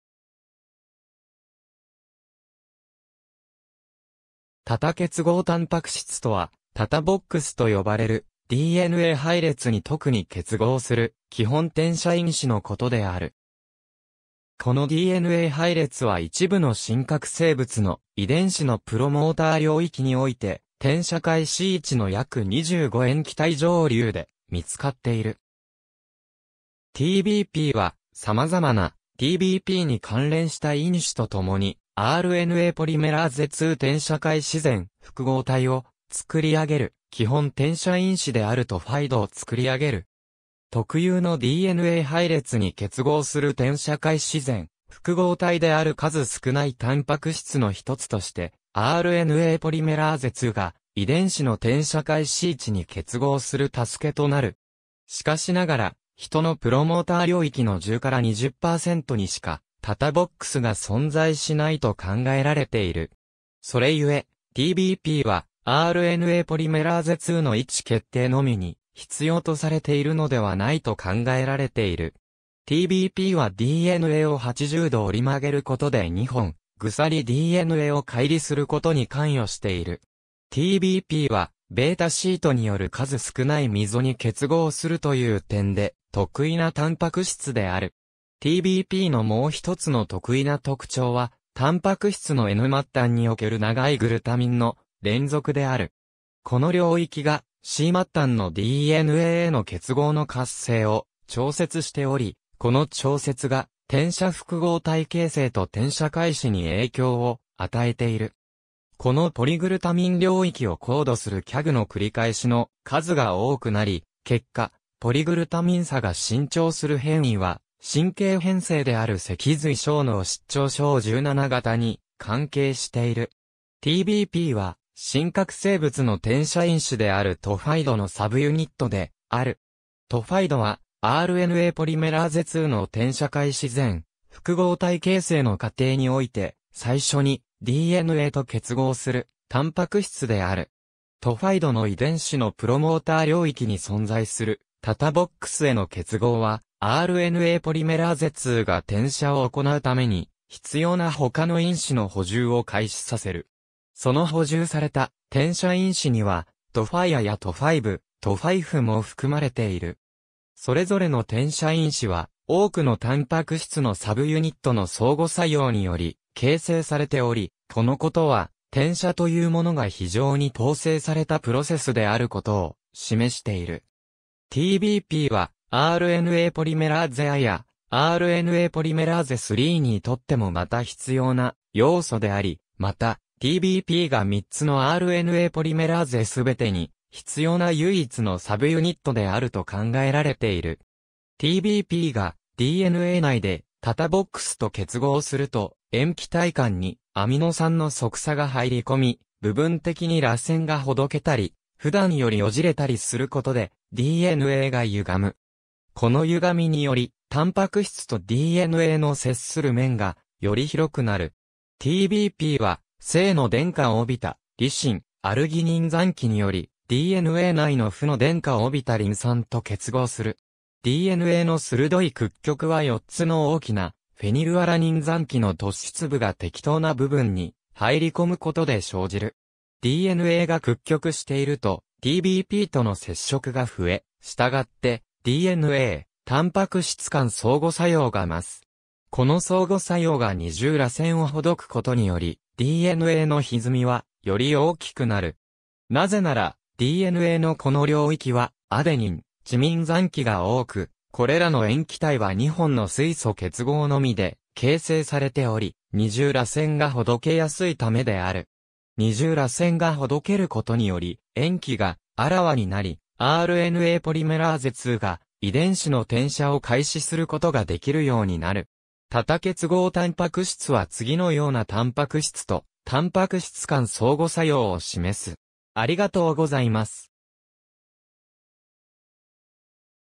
TATA結合タンパク質
TATA結合タンパク質（ターターけつごうタンパクしつ、英: TATA-binding protein、TBP）とは、TATAボックスと呼ばれるDNA配列に特に結合する基本転写因子のことである。
このDNA配列は一部の真核生物の遺伝子のプロモーター領域において転写開始位置の約25塩基対上流で見つかっている。
TBPは、様々なTBPに関連した因子とともにRNAポリメラーゼII転写開始前複合体を作り上げる基本転写因子であるTFIIDを作り上げる。
特有のDNA配列に結合する転写開始前複合体である数少ないタンパク質の1つとして、RNAポリメラーゼIIが遺伝子の転写開始位置に結合する助けとなる。
しかしながら、ヒトのプロモーター領域の10-20%にしかTATAボックスが存在しないと考えられている。
それゆえ、TBPはRNAポリメラーゼIIの位置決定のみに必要とされているのではないと考えられている。
TBPはDNAを80°折り曲げることで二本鎖DNAを解離することに関与している（多くのA-T間の結合はより容易にほどける）。
TBPはβシートによる数少ない溝に結合するという点で、特異なタンパク質である。
TBPのもう1つの特異な特徴は、タンパク質のN末端における長いグルタミンの連続である。
この領域がC末端のDNAへの結合の活性を調節しており、この調節が転写複合体 (transcription complex) 形成と転写開始に影響を与えている。
このポリグルタミン領域をコードするCAGの繰り返しの数が多くなり、結果、ポリグルタミン鎖が伸長する変異は、神経変性である脊髄小脳失調症17型 (SCA17) に関係している。
TBPは真核生物の転写因子であるTFIIDのサブユニットである。
TFIIDはRNAポリメラーゼIIの転写開始前複合体形成の過程において最初にDNAと結合するタンパク質である。
TFIIDの遺伝子のプロモーター領域に存在するTATAボックスへの結合は、RNAポリメラーゼIIが転写を行うために必要なほかの因子の補充を開始させる。
その補充された転写因子にはTFIIAやTFIIB、TFIIFも含まれている。
それぞれの転写因子は多くのタンパク質のサブユニットの相互作用により形成されており、このことは、転写というものが非常に統制されたプロセスであることを示している。
TBPはRNAポリメラーゼIやRNAポリメラーゼIIIにとってもまた必要な要素であり、また、TBPが3つのRNAポリメラーゼ全てに必要な唯一のサブユニットであると考えられている。
TBPがDNA内でTATAボックスと結合すると、塩基対間にアミノ酸の側鎖が入り込み、部分的に螺旋がほどけたり、普段よりよじれたりすることでDNAがゆがむ。
このゆがみにより、タンパク質とDNAの接する面がより広くなる。
TBPは、正の電荷を帯びたリシン、アルギニン残基により、DNA内の負の電荷を帯びたリン酸と結合する。
DNAの鋭い屈曲は4つの大きなフェニルアラニン残基の突出部が適当な部分に入り込むことで生じる。
DNAが屈曲していると、TBPとの接触が増え、したがってDNA-タンパク質間相互作用 (DNA-protein interaction) が増す。
この相互作用が二重らせんをほどくことによりDNAのひずみはより大きくなる。
なぜならDNAのこの領域はアデニン、チミン残基が多く、これらの塩基対は2本の水素結合のみで形成されており、二重らせんがほどけやすいためである。
二重らせんがほどけることにより塩基があらわになり、RNAポリメラーゼIIが遺伝子の転写を開始することができるようになる。
TATA結合タンパク質は次のようなタンパク質とタンパク質間相互作用を示す。